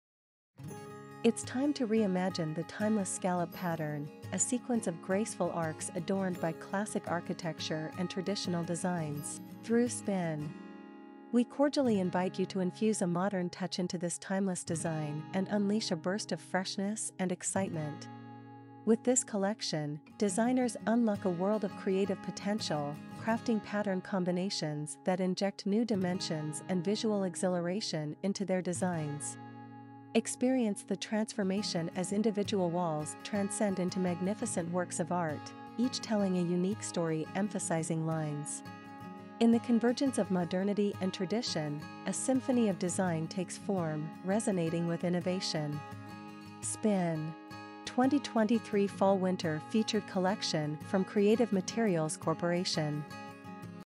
Spin | 2023 Fall Winter Feature Collection
Spin ceramic wall tile enables designers to create pattern combinations that add new dimensions and visual excitement to design schemes. Individual interior walls become works of art! Each project becomes a captivating journey, an experience waiting to unfold as the elements intertwine and evolve.